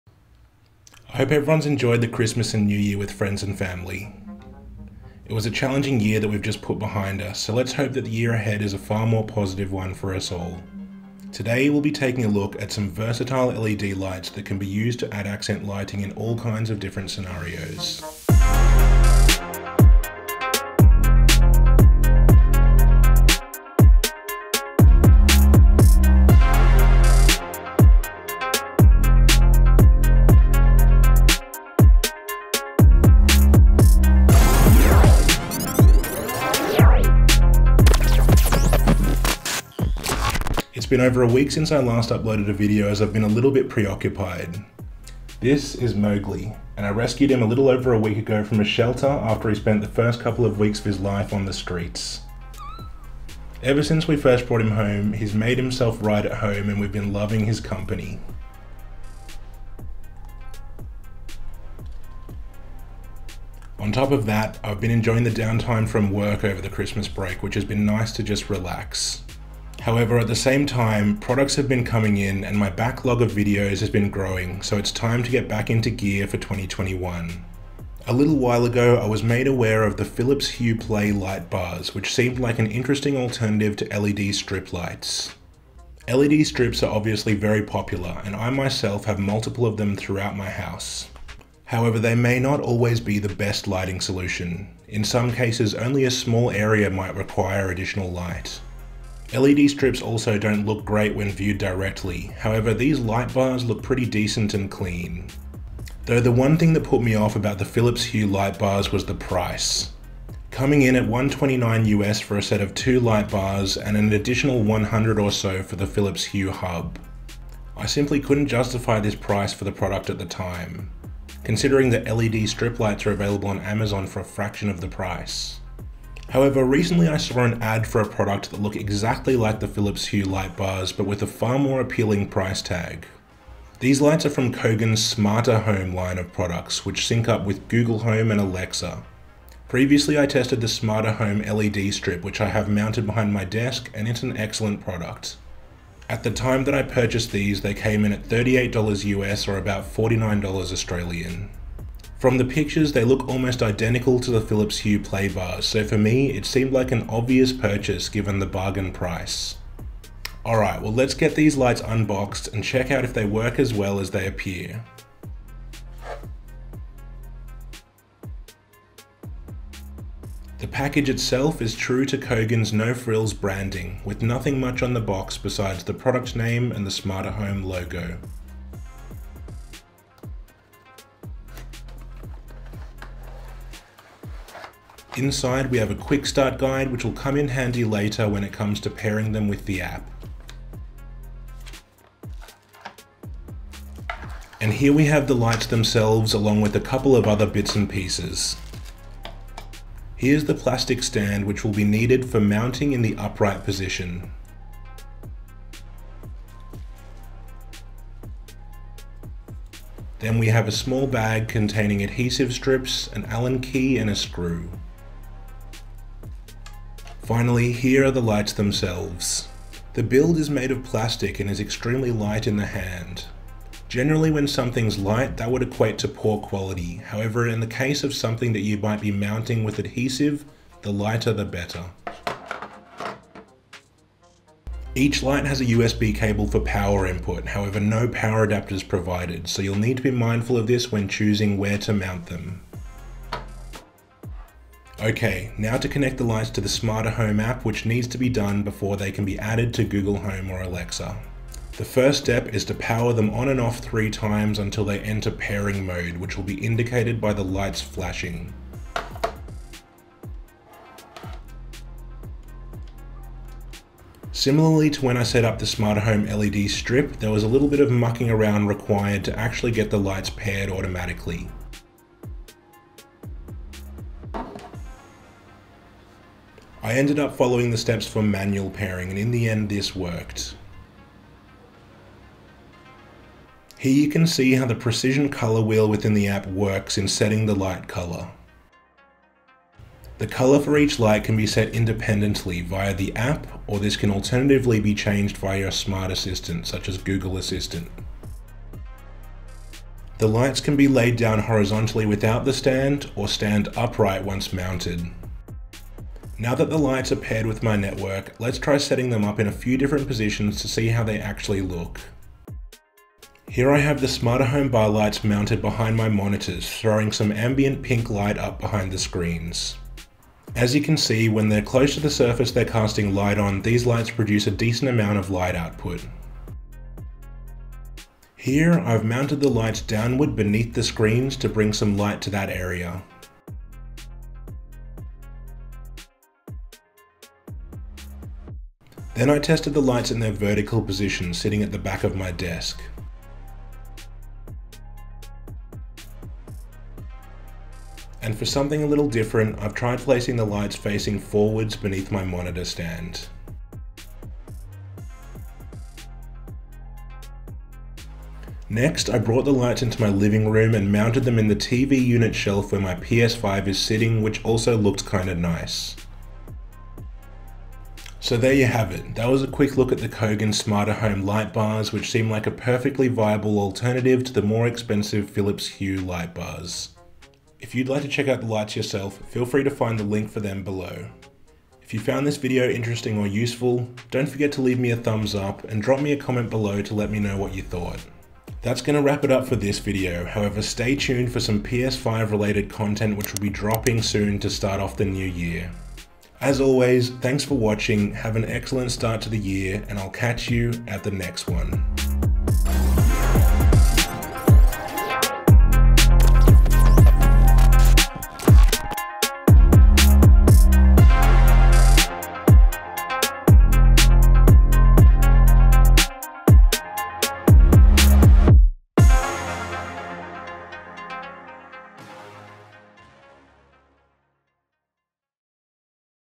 Do they suck? CHEAP Philips Hue Light Bar Alternative! (Kogan SmarterHome)
My camera gear:
Lens 1
Lens 2
Key & Fill Lights
Rim light/Accent light

0:00 - Intro
1:18 - Philips Hue Light Bar
2:06 - Kogan SmarterHome Light Bar
2:40 - Unboxing & setup
5:06 - Testing the lights
6:08 - Outro